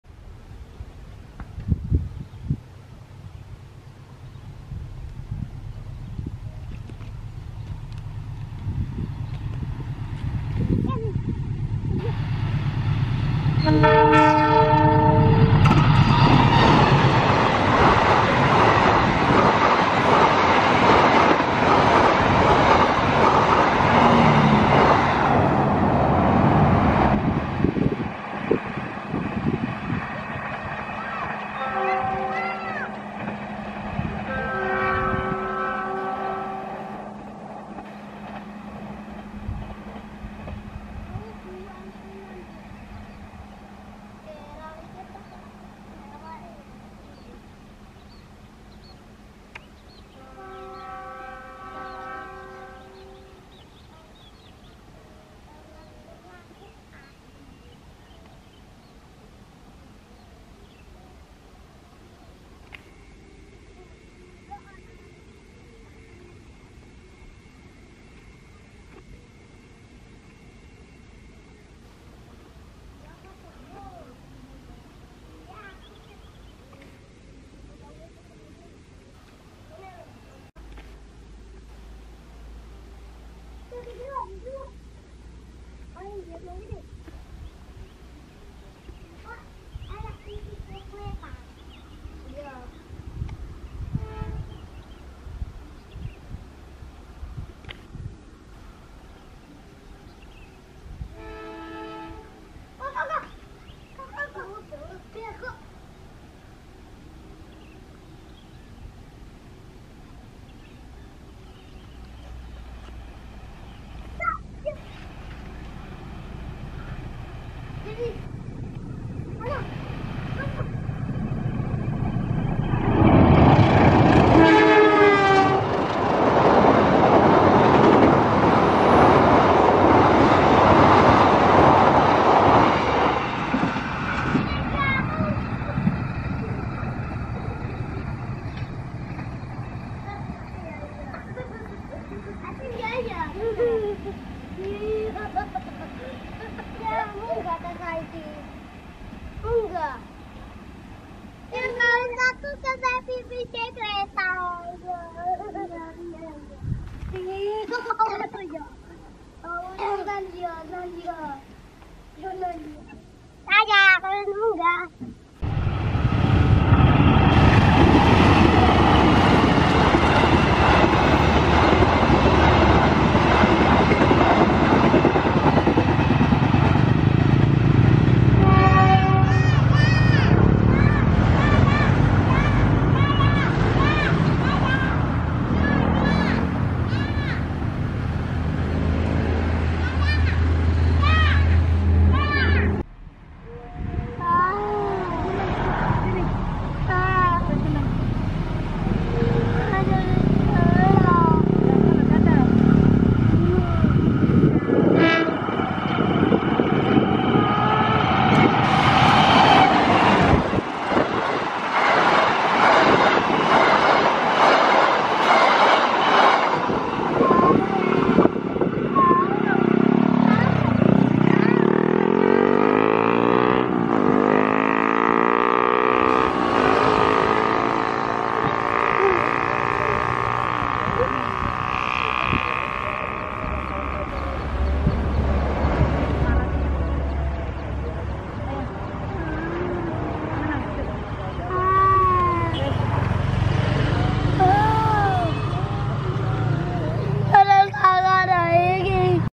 Kereta Api Kertanegara Ngebut Melinta Di Jembatan KA Kali Batan Purwoasri - Kediri
Hallo BossQ..Kami Hadirkan Beberapa Kereta api yang Ngebut saat melintas Di jembatan dan perlintasan tanpa Palang Dijalur Kantong...selamat menonton

Kereta Api Kertanegara Ngebut Melintas di Jembatan KA Kali Batan